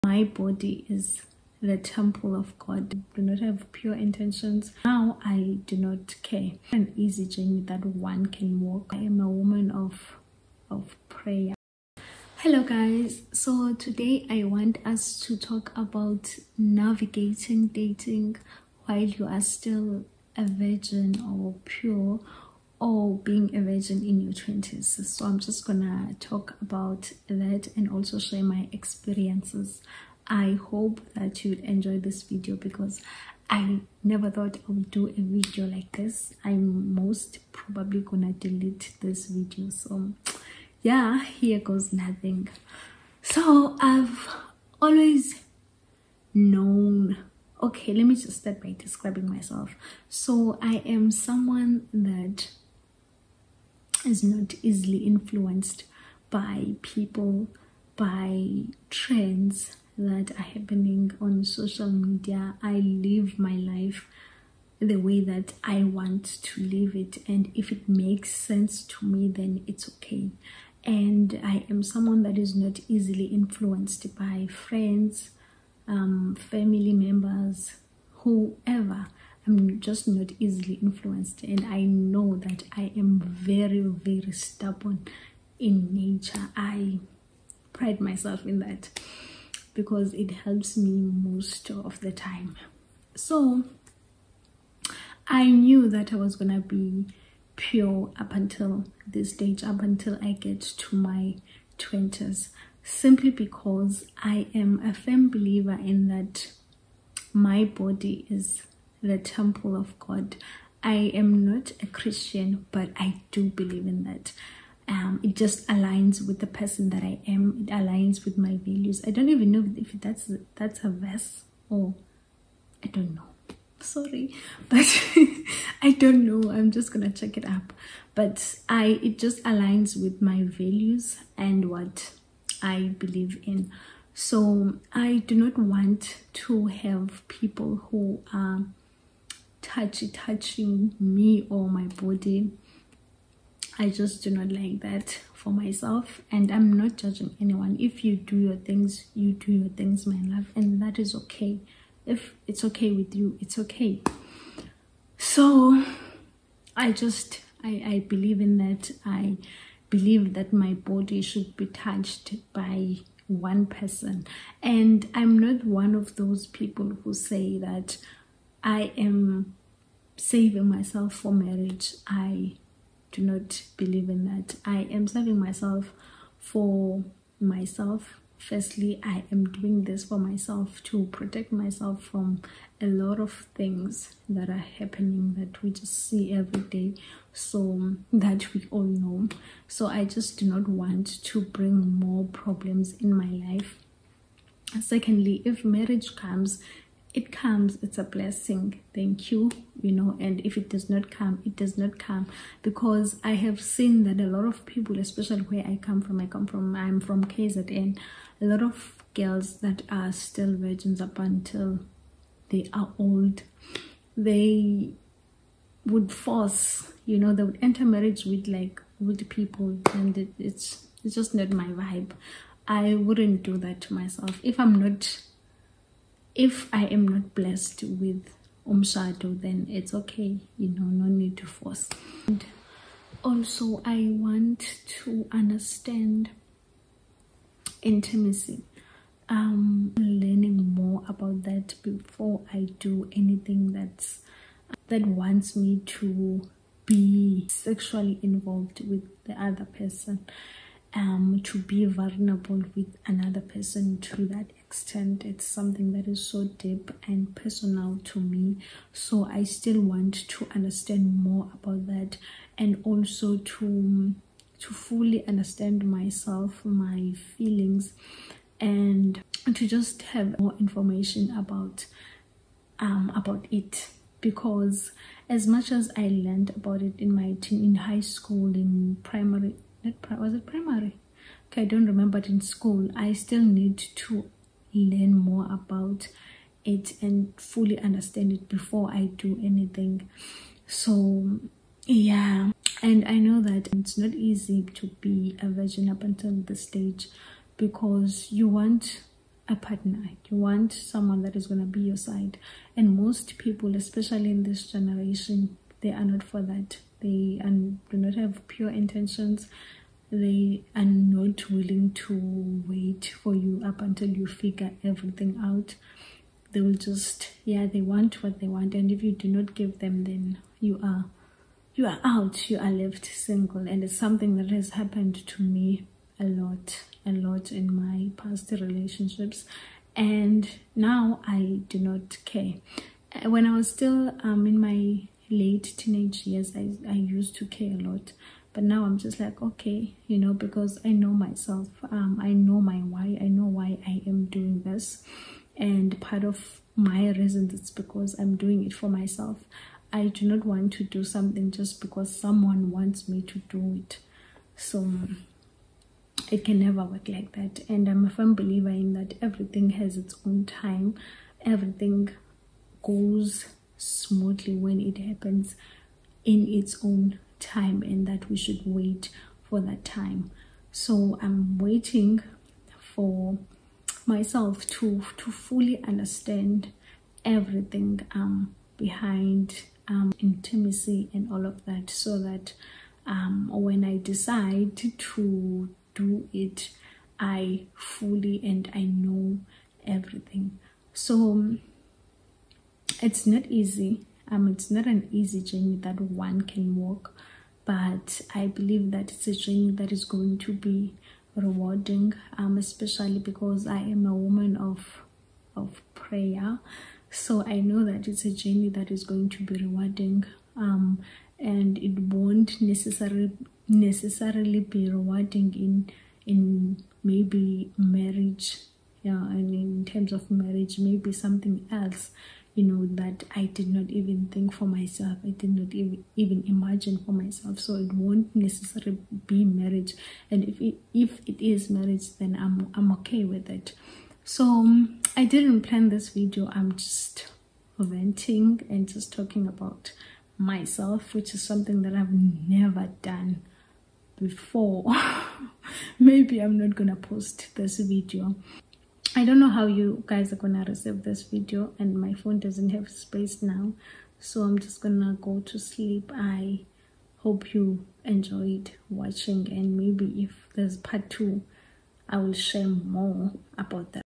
Being a virgin in your 20s/ Navigating relationships while you’re still pure|South African YouTuber
Let’s talk about being a virgin in your 20s and navigating relationships while you’re still pure.